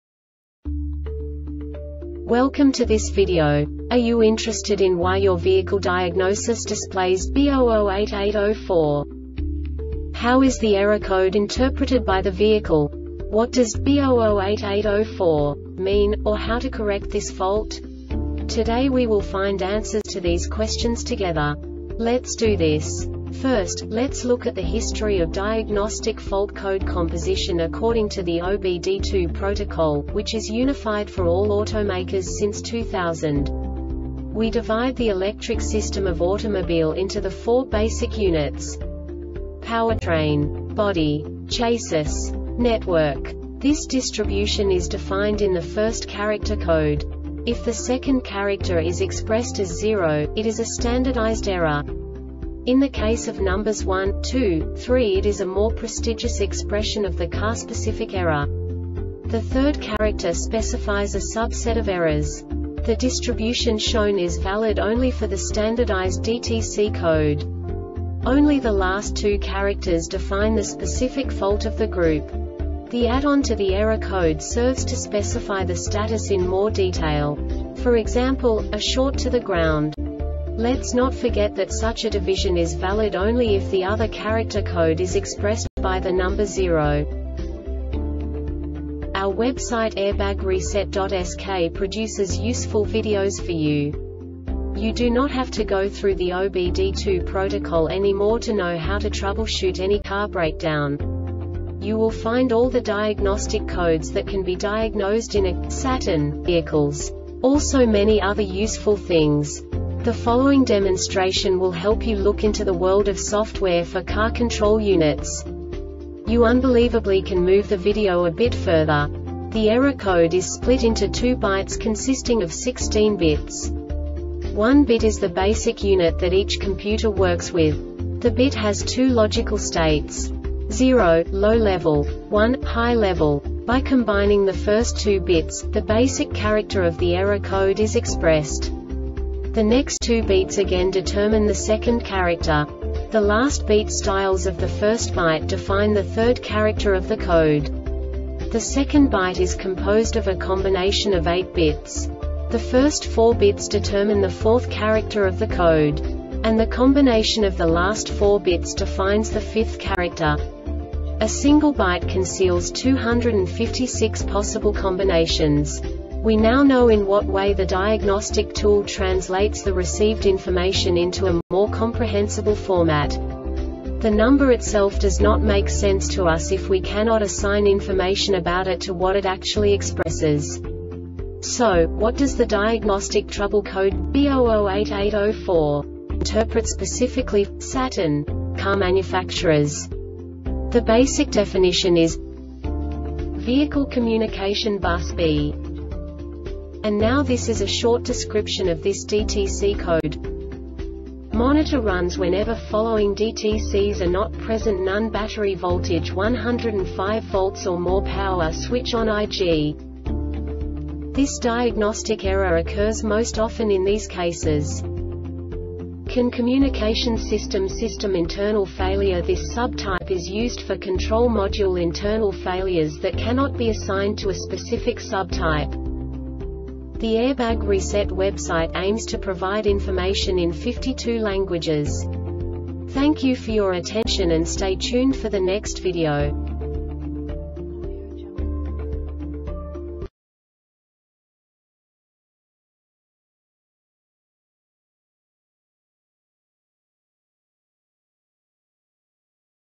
DTC Saturn B0088-04 Short Explanation
Contents:
0:21 Basic DTC analysis according to OBD2 protocol standard.
1:48 Insight into programming
2:58 A diagnostically understandable description of the Diagnostic Trouble Code
3:05 Basic error definition

Make:
Saturn
Code:
B0088 04
Definition:
Vehicle Communication Bus "B"
Description:
Cause:
 CAN communication system
Failure Type: